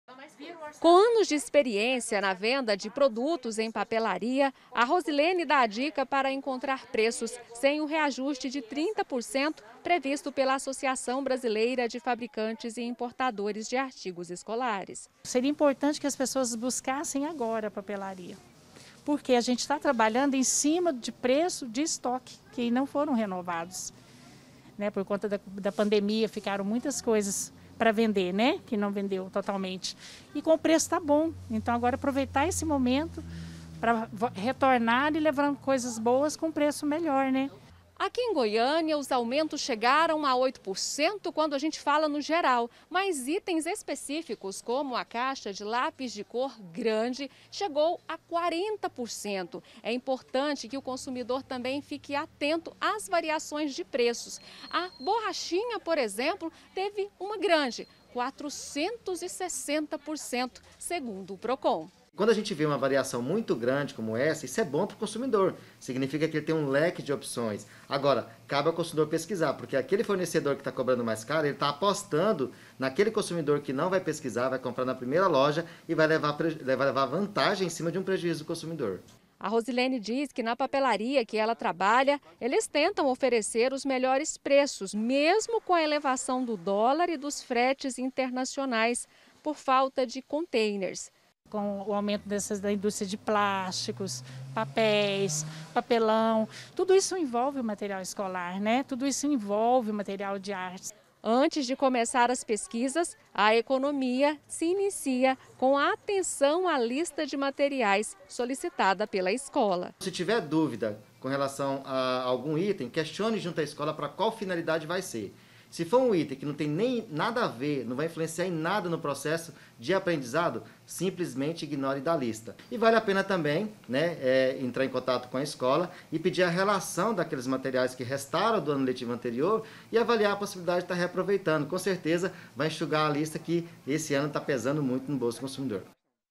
DÓI NO BOLSO: MATERIAL ESCOLAR PESA NO ORÇAMENTO DAS FAMÍLIAS
VAI COMPRAR MATERIAL ESCOLAR? ENTÃO SE PREPARE. NO GERAL, ELES SUBIRAM CERCA DE OITO POR CENTO. JÁ A VARIAÇAÕ DE ALGUNS ITENS DE UMA PAPELARIA PRA OUTRA, PODE PASSAR DOS QUATROCENTOS POR CENTO!